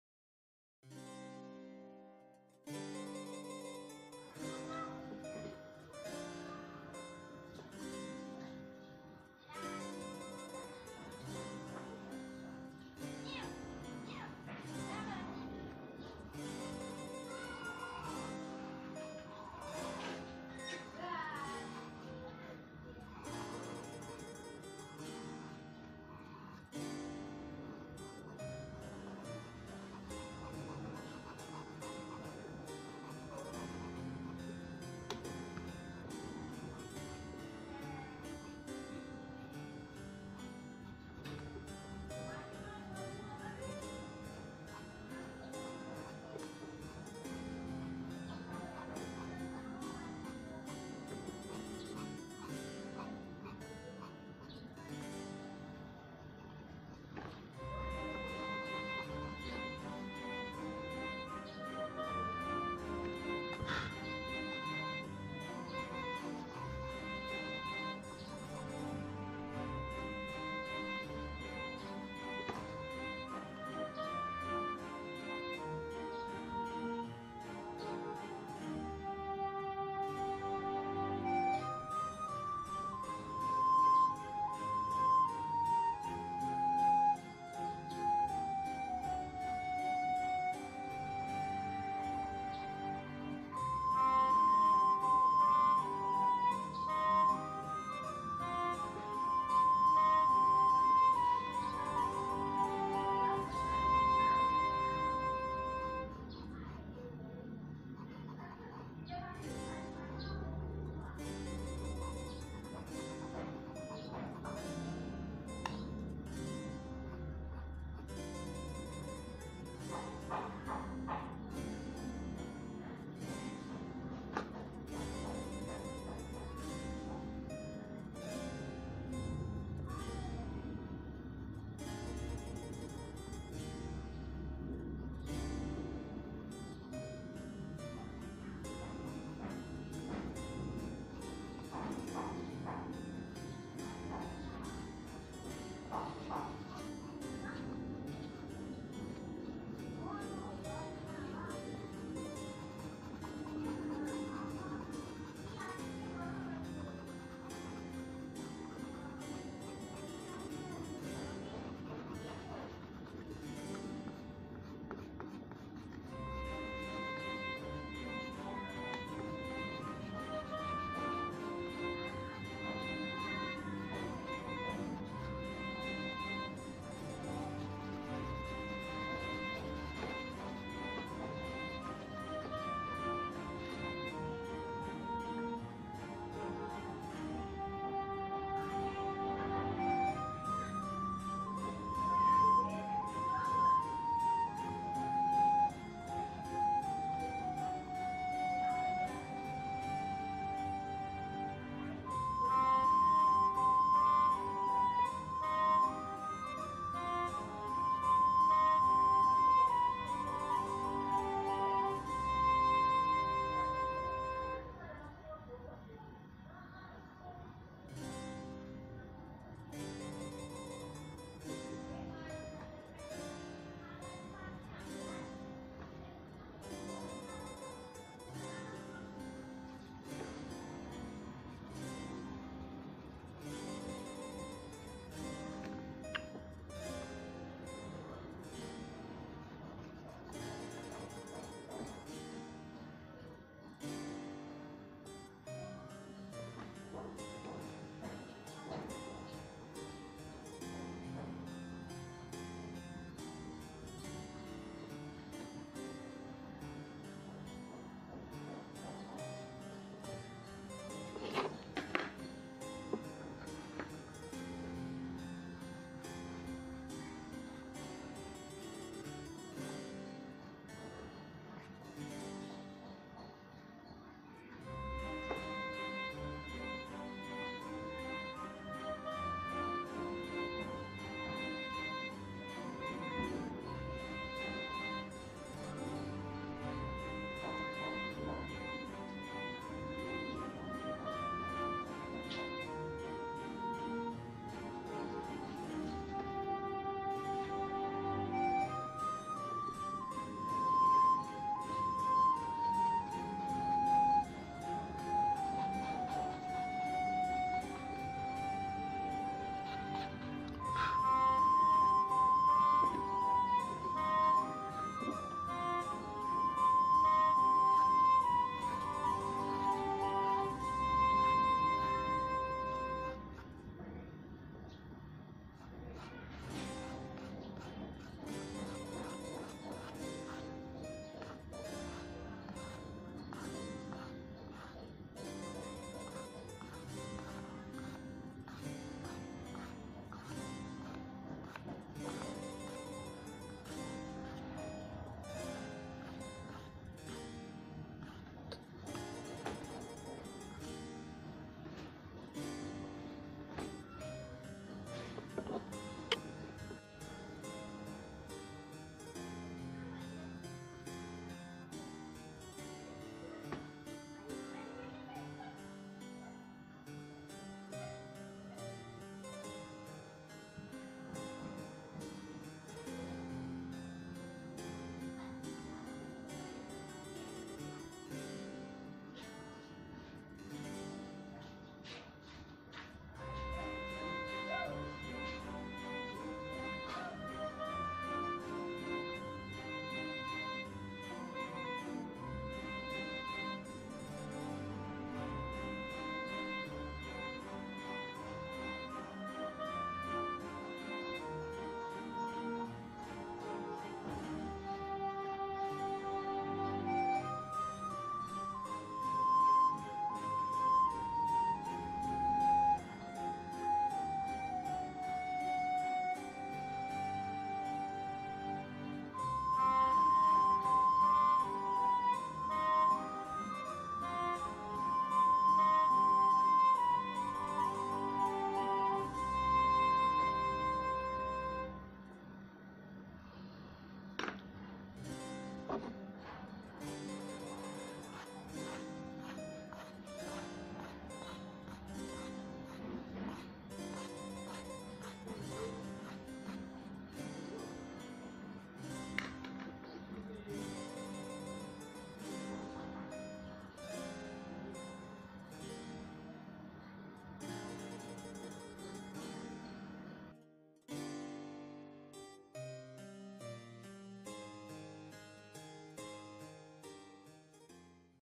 drawing girl eyes step by step || sad drama
hai..i gonna show you the way i draw the eyes step by step..
hai friend..lets draw with me
Yuk menggambar wajah cantik dengan versi lambat..lets draw beautiful face step by step

material needed :
Pencil conte
Sketch book